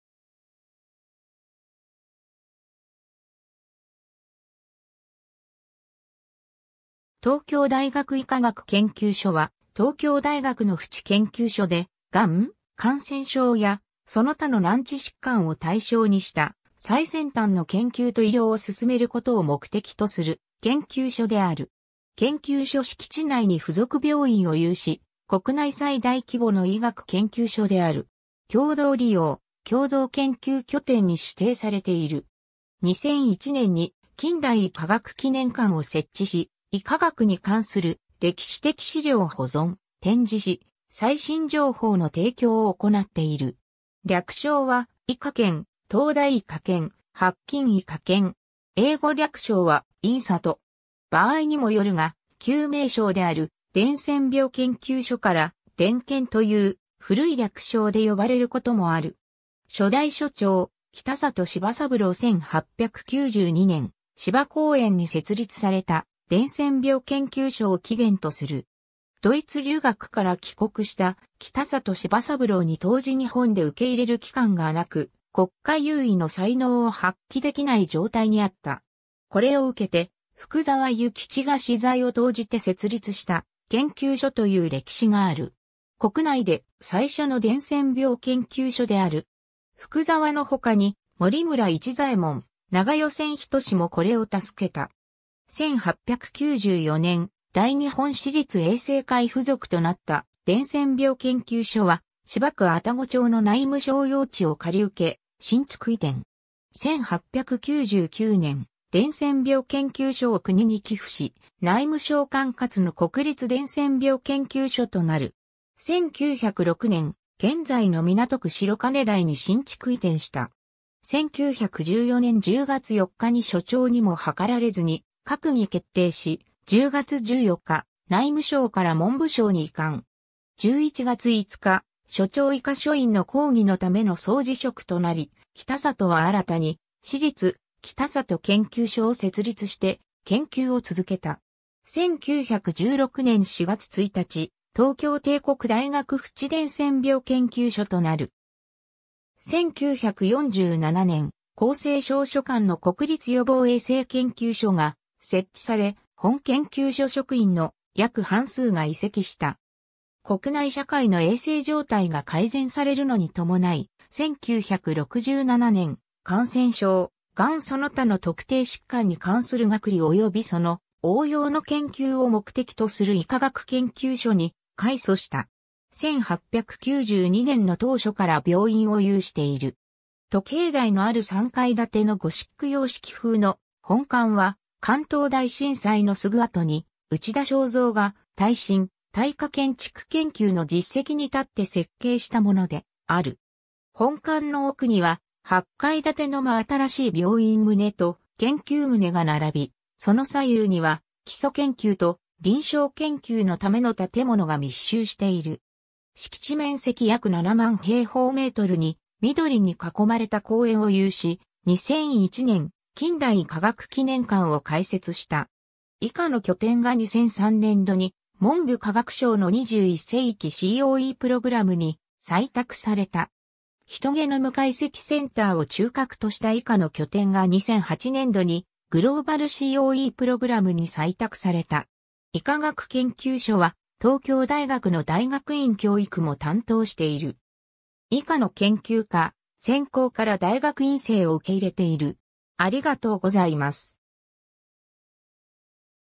東京大学医科学研究所
白金_(東京都港区)
東京大学医科学研究所（とうきょうだいがくいかがくけんきゅうじょ、英: The Institute of Medical Science, The University of Tokyo）は、東京大学の附置研究所で、がん、感染症やその他の難治疾患を対象にした最先端の研究と医療を進めることを目的とする研究所である。
研究所敷地内に附属病院を有し、国内最大規模の医学研究所である。
 共同利用・共同研究拠点に指定されている（「基礎・応用医科学の推進と先端医療 の実現を目指した医科学共同研究拠点」）。
2001年に近代医科学記念館を設置し、医科学に関する歴史的資料を保存・展示し、最新情報の提供を行っている。
 略称は「医科研」「東大医科研」「白金医科研」。
英語略称は「IMSUT」。
場合にもよるが、旧名称である伝染病研究所から「伝研」という古い略称で呼ばれることもある。
 初代所長・北里柴三郎 1892年（明治25年）、芝公園に設立された、伝染病研究所を起源とする。
ドイツ留学から帰国した北里柴三郎に当時日本で受け入れる機関がなく、国家有為の才能を発揮できない状態にあった。
これを受けて、福澤諭吉が私財を投じて設立した研究所という歴史がある。
国内で最初の伝染病研究所である。
福澤の他に森村市左衛門、長與專齋もこれを助けた。
1894年（明治27年）、大日本私立衛生会附属となった伝染病研究所は芝区愛宕町の内務省用地を借り受け、新築移転。
1899年（明治32年）、伝染病研究所を国に寄付し、内務省管轄の国立伝染病研究所となる。
1906年（明治39年）、現在の港区白金台に新築移転した。
 1914年（大正3年）10月4日に所長にもはかられずに閣議決定し、10月14日、内務省から文部省に移管（勅令）。
11月5日所長以下所員の抗議のための総辞職となり、北里は新たに私立「北里研究所」を設立して研究を続けた。
1916年（大正5年）4月1日、東京帝国大学附置伝染病研究所となる。
 1947年（昭和22年）、厚生省所管の国立予防衛生研究所（現：国立感染症研究所）が設置され、本研究所職員の約半数が移籍した。
 国内社会の衛生状態が改善されるのに伴い、1967年（昭和42年）、「感染症・がんその他の特定疾患に関する学理及びその応用の研究」を目的とする医科学研究所に改組した。
 1892年の当初から病院を有している。
 時計台のある3階建てのゴシック様式風の本館は、関東大震災（1923年（大正12年））のすぐあとに、内田祥三（のちの総長）が耐震・耐火建築研究の実績に立って設計したものである。
本館の奥には、8階建ての真新しい病院棟と研究棟が並び、その左右には基礎研究と臨床研究のための建物が密集している。
敷地面積約7万平方メートルに、緑に囲まれた公園を有し、2001年、近代医科学記念館を開設した。
 以下の拠点が2003年度に文部科学省の21世紀COEプログラムに採択された。
 ヒトゲノム解析センターを中核とした以下の拠点が2008年度にグローバルCOEプログラムに採択された。
 医科学研究所は東京大学の大学院教育も担当している。
以下の研究科・専攻から大学院生を受け入れている。